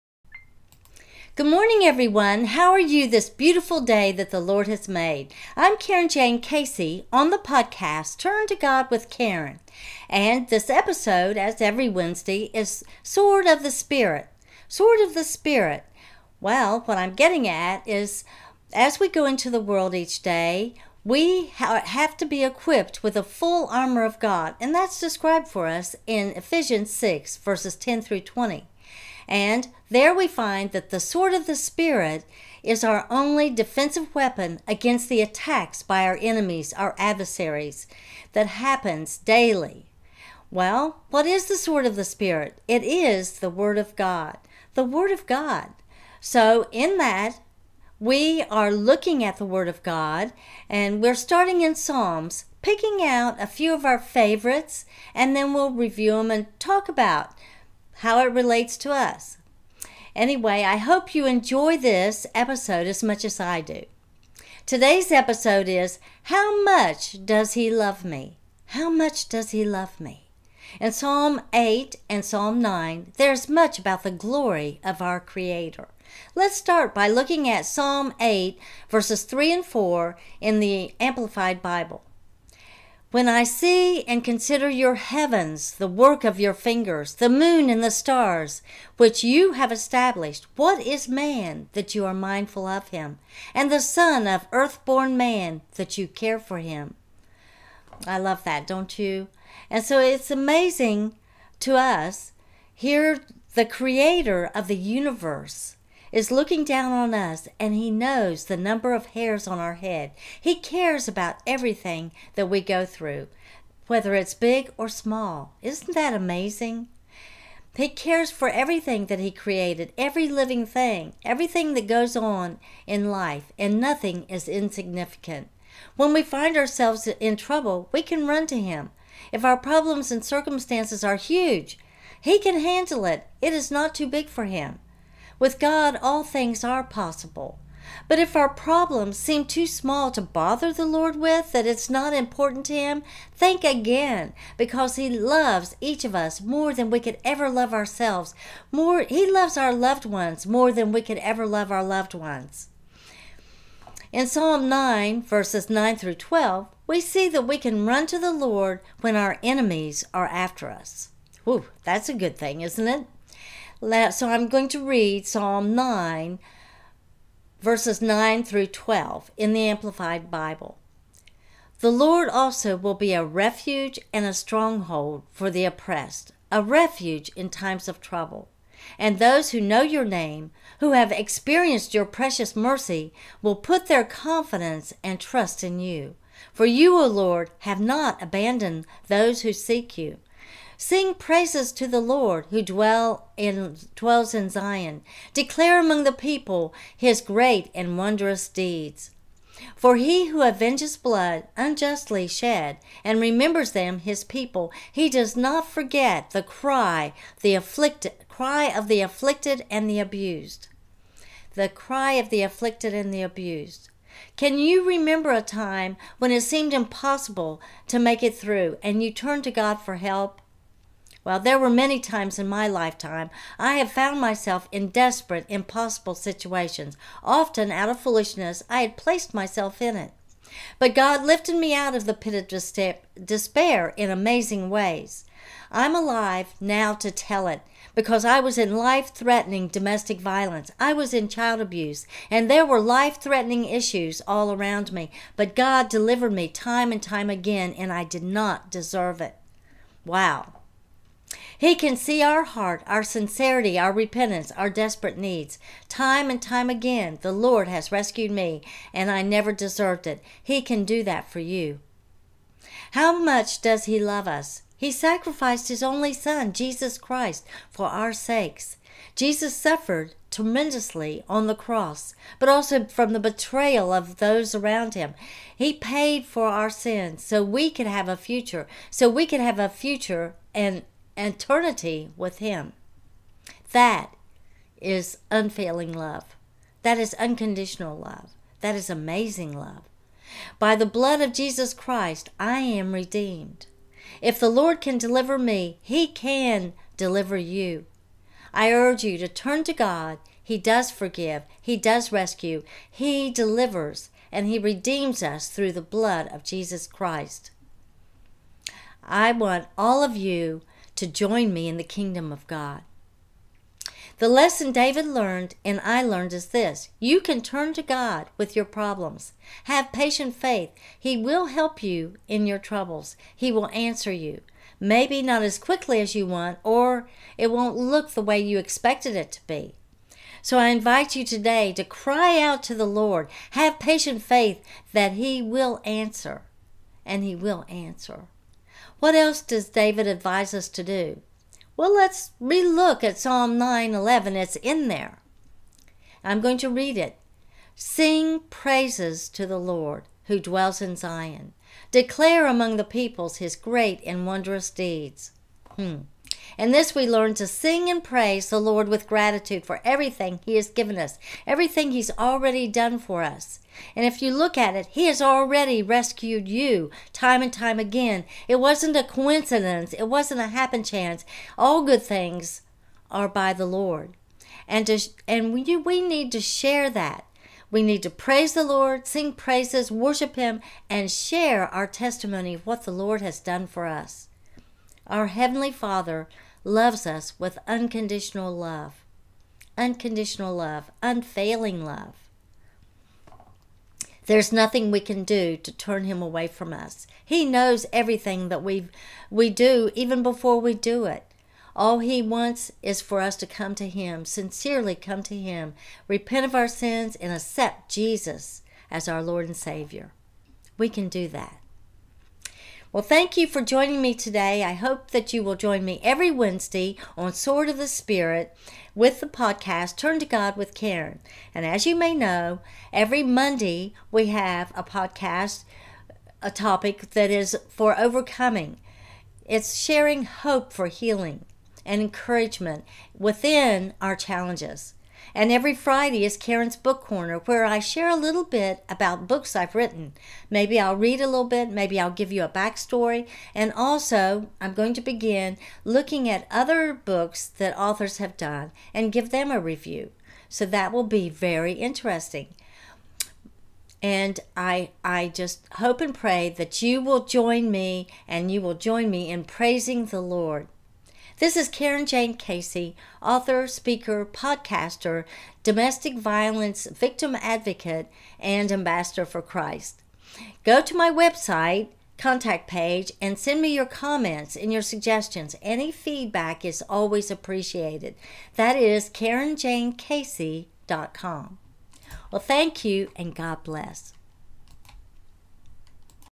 How Much Does He Love Me?
Sword of the Spirit: “How Much Does He Love Me?”
In Psalms 8:3-4 and 9:9-12 we have a picture of the Lord’s unfailing love.

The Creator of the Universe sacrificed His only Son, Jesus, for our salvation! When we sincerely turn to our Heavenly Father, He forgives us, rescues us from our enemies, and redeems!